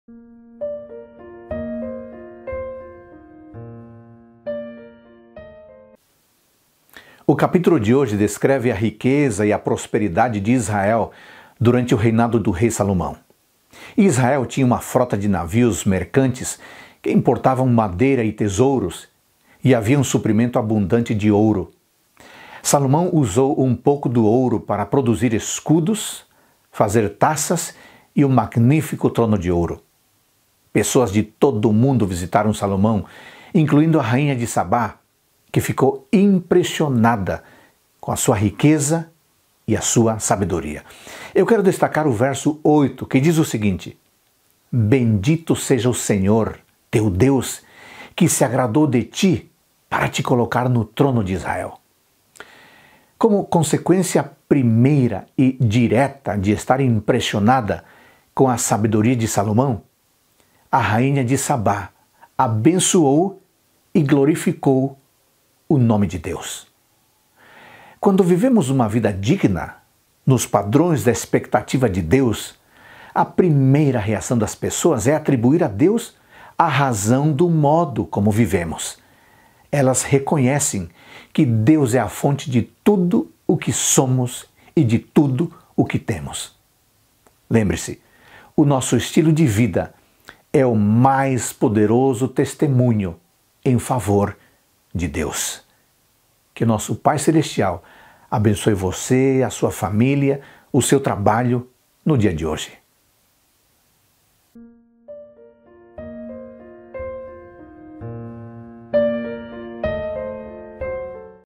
1 Reis 10 - Reavivados por sua Palavra
Quando vivemos uma vida digna, a primeira reação das pessoas é atribuir a Deus a razão do modo como vivemos. Nosso estilo de vida é o mais poderoso testemunho em favor de Deus.